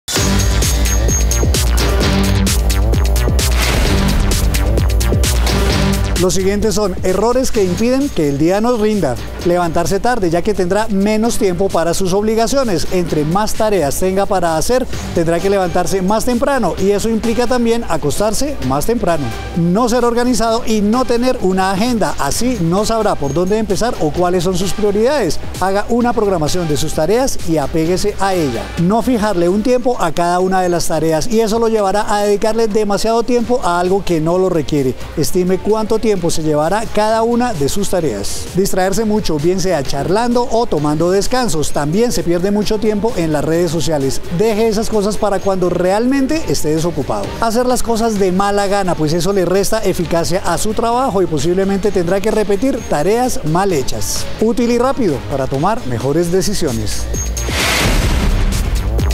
Cinco errores que seguro está cometiendo si dice “es que el día no me alcanza’
Muchas veces es más un tema de planeación. Aquí le contamos los pecados que lo pueden volver menos productivo.